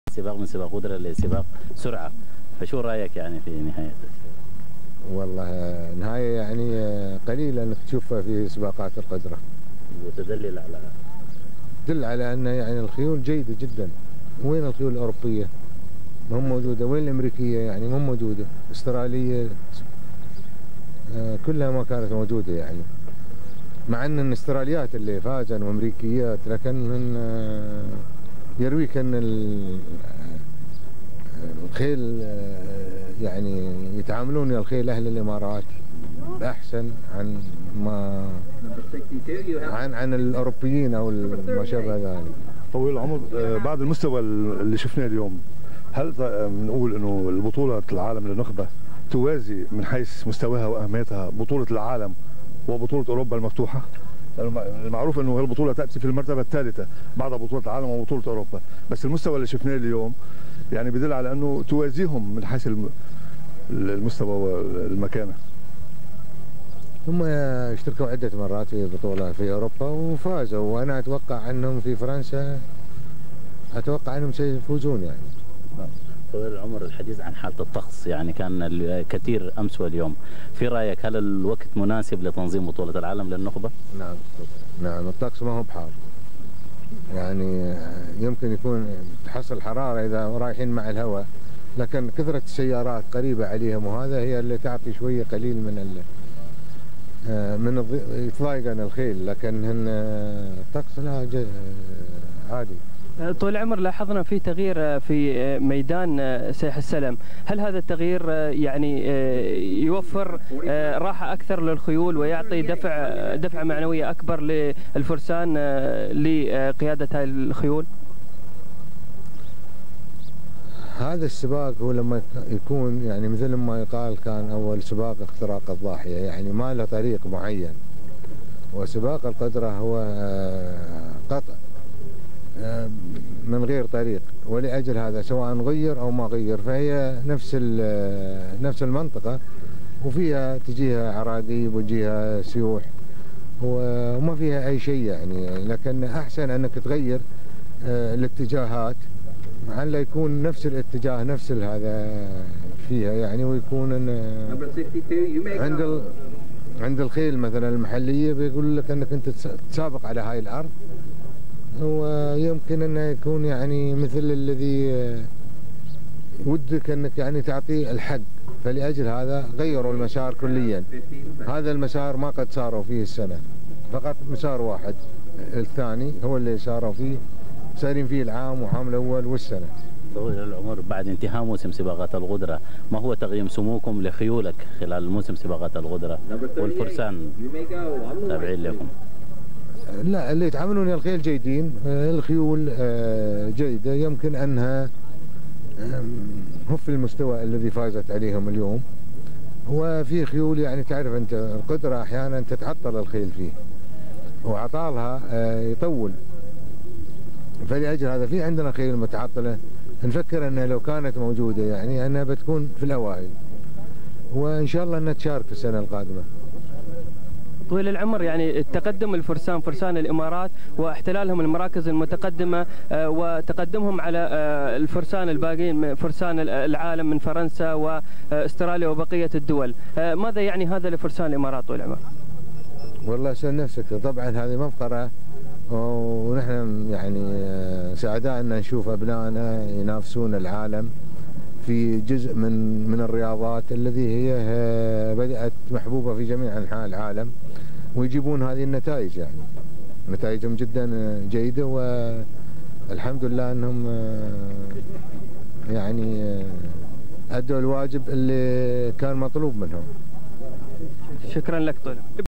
سباقات القدره - خيول - الشيخ حمدان بن راشد آل مكتوم - أرشيف حسين العوضي 📽️
من الارشيف التلفزيوني الخاص 📺 قناة دبي
تصريح سمو الشيخ حمدان بن راشد آل مكتوم.. طيب الله ثراه خلال بطولة العالم الثانية للنخبة.. لسباقات القدرة بسيح السلم بدبي
عرف عن سموه رحمه الله تعلقه وحبه واقتنائه لاجود سلالات الخيول ومشاركاته في مختلف السباقات العالمية... خبرته الكبيره في فن التعامل مع الخيل ومراحل تدريبها
ودائما مع الذكريات نبقى 🌹
حسين العوضي 📽️